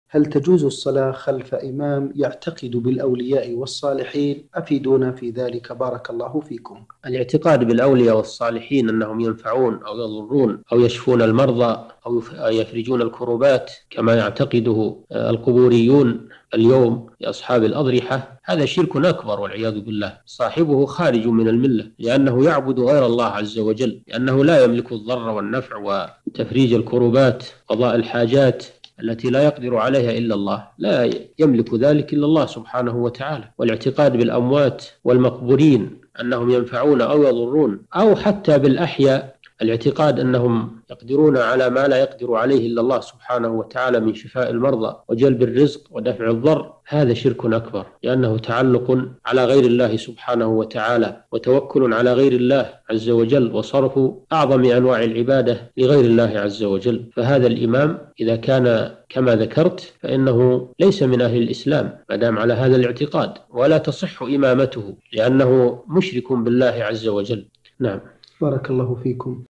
هل تجوز الصلاة خلف إمام يعتقد بالاولياء والصالحين؟ الشيخ صالح بن فوزان الفوزان
نص السؤال:
هل تجوز الصلاة خلف إمام يعتقد بالاولياء والصالحين أفيدونا في ذلك بارك الله فيكم؟

السيرة الذاتية :
للاشتراك في قناة الشيخ صالح الفوزان

رئيس الفريق
لمشروع كبار العلماء